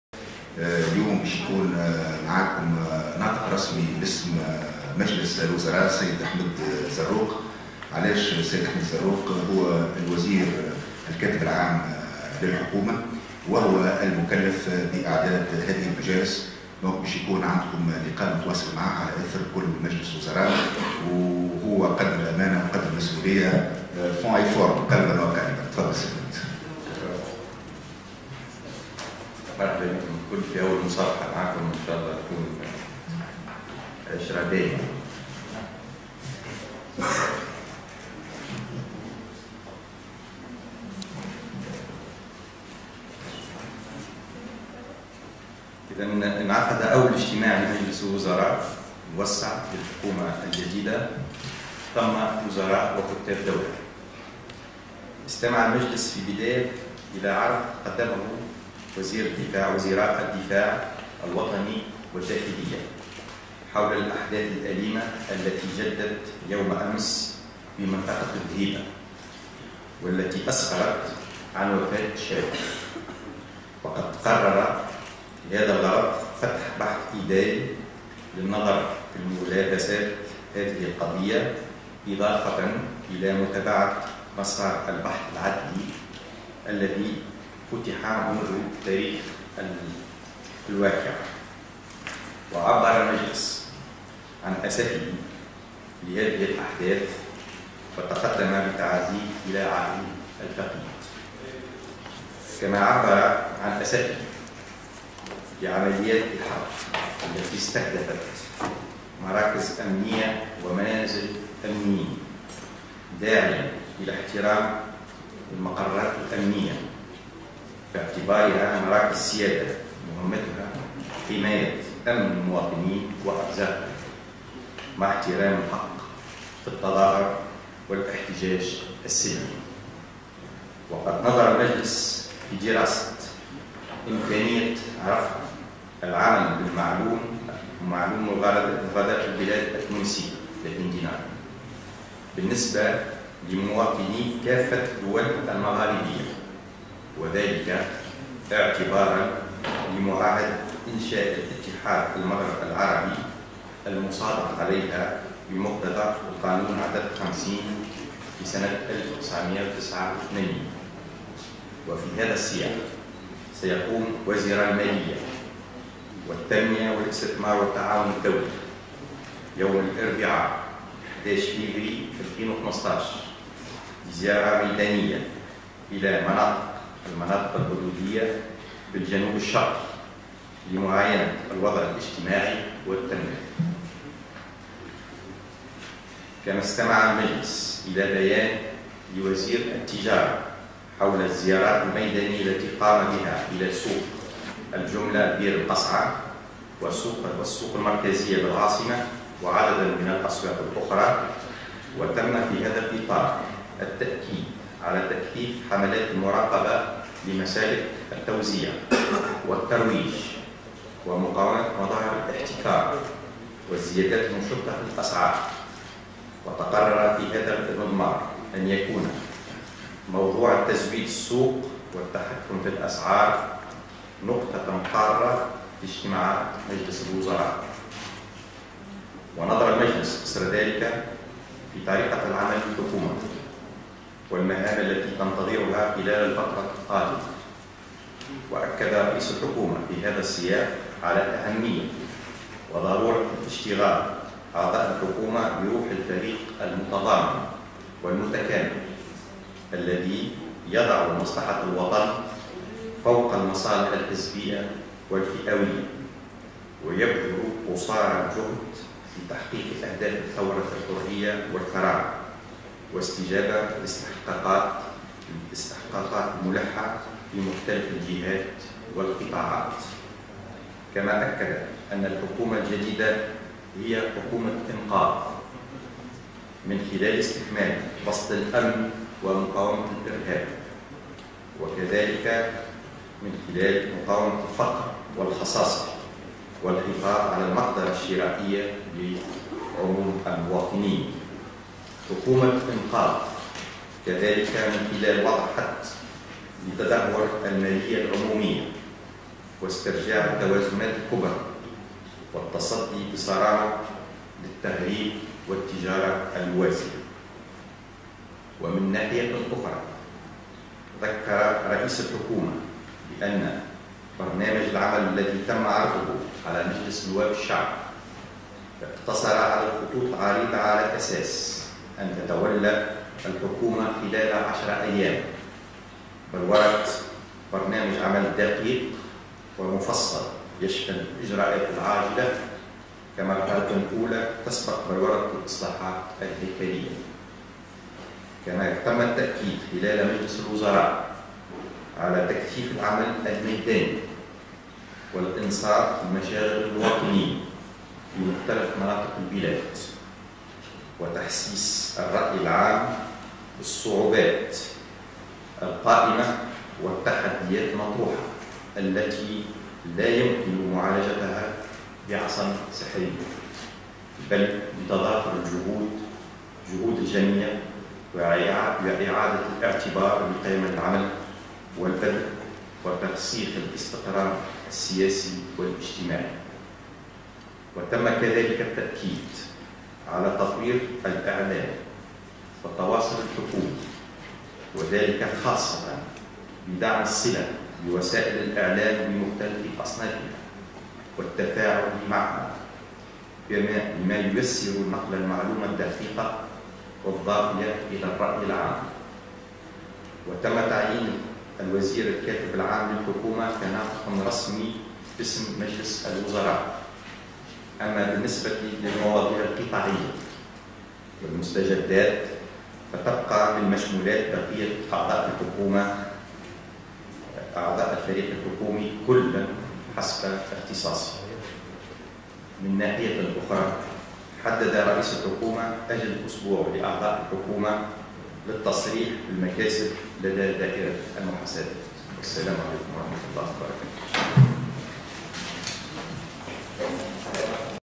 دراسة إمكانية تعليق العمل بمعلوم مغادرة البلاد التونسية بالنسبة لمواطني كافة الدول المغاربية
أشرف رئيس الحكومة الحبيب الصّيد بعد ظهر اليوم بقصر الحكومة بالقصبة على انعقاد أول اجتماع لمجلس وزراء موسّع للحكومة الجديدة ضمّ الوزراء وكتاب الدولة.

واستمع المجلس في البداية إلى عرض قدّمه وزيرا الدفاع الوطني والداخلية حول الأحداث الأليمة التي جدّت يوم أمس بمنطقة ذهيبة، والتي أسفرت عن وفاة شاب. وعبّر المجلس عن أسفه لهذه الأحداث وتقدّم بتعازيه إلى عائلة الفقيد، كما قرر فتح بحث إداري للنظر في ملابسات هذه القضية إضافة إلى متابعة البحث العدلي الذي فـُتح منذ تاريخ الواقعة.

كما عبّر عن أسفه لعمليّات الحرق التي استهدفت مراكز أمنيّة ومنازل أمنيّين داعيا إلى احترام المقرّات الأمنيّة باعتبارها مراكز سيادة مهمّتها حماية أمن المواطنين وممتلكاتهم مع احترام حقّ التظاهر والاحتجاج السلمي.

وقرّر مجلس الوزراء دراسة إمكانية تعليق العمل بمعلوم مغادرة البلاد التونسية بالنسبة لمواطني كافة الدول المغاربية وذلك تماشيا مع معاهدة إنشاء اتحاد المغرب العربي المصادق عليها بمقتضى القانون عدد 50 لسنة 1989.

وسيقوم وزير المالية ووزير التنمية والاستثمار والتعاون الدولي، يوم الأربعاء 11 فيفري 2015 بزيارة ميدانية إلى المناطق الحدوديّة بالجنوب الشرقي لمعاينة الوضع الاجتماعي والتنموي.

كما استمع المجلس إلى بيان لوزير التجارة حول الزيارات الميدانيّة التي قام بها إلى سوق الجملة ببئر القصعة والسوق المركزية بالعاصمة وعدد من الأسواق الأخرى. وتمّ في هذا الإطار التأكيد على تكثيف حملات المراقبة لمسالك التوزيع والترويج ومقاومة مظاهر الاحتكار والزيادات المشطة في الأسعار. وتقرر في هذا السياق أن يكون موضوع تزويد السوق والتحكم في الأسعار نقطة قارة في اجتماعات مجلس الوزراء.

ونظر المجلس إثر ذلك في طريقة عمل الحكومة والمهام التي تنتظرها خلال الفترة القادمة.

وأكد رئيس الحكومة في هذا المجال على أهمية وضرورة اشتغال أعضاء الحكومة بروح الفريق المتضامن والمتكامل الذي يضع المصلحة الوطنية فوق المصالح الحزبية والفئوية، ويبذل قصارى الجهد لتحقيق أهداف ثورة الحرية والكرامة والاستجابة للاستحقاقات الملحة في مختلف الجهات والقطاعات.

كما أكد أنّ الحكومة الجديدة هي حكومة إنقاذ من خلال استكمال بسط الأمن ومقاومة الإرهاب وكذلك من خلال مقاومة الفقر والخصاصة والحفاظ على المقدرة الشرائيّة لعموم المواطنين ومن خلال وضع حدّ لتدهور المالية العمومية واسترجاع التوازنات الكبرى، والتصدّي بصرامة للتهريب والتجارة الموازية.

ومن ناحية أخرى ذكـّر رئيس الحكومة بأن برنامج العمل الذي تمّ عرضه على مجلس نواب الشعب اقتصر على الخطوط العريضة على أساس أن تتولى الحكومة خلال 10 أيام بلورة برنامج عمل دقيق ومفصّل يشمل الإجراءات العاجلة كمرحلة أولى تسبق بلورة  الإصلاحات الهيكليّة.

كما تمّ التأكيد خلال مجلس الوزراء على تكثيف العمل الميداني والانصات لمشاغل المواطنين في مختلف مناطق البلاد، وتحسيس الرأي العام بالصعوبات القائمة والتحديات المطروحة التي لا يمكن معالجتها بعصا سحريّة بل بتضافر جهود الجميع وإعادة الاعتبار لقيم العمل والبذل وترسيخ الاستقرار السياسي والاجتماعي.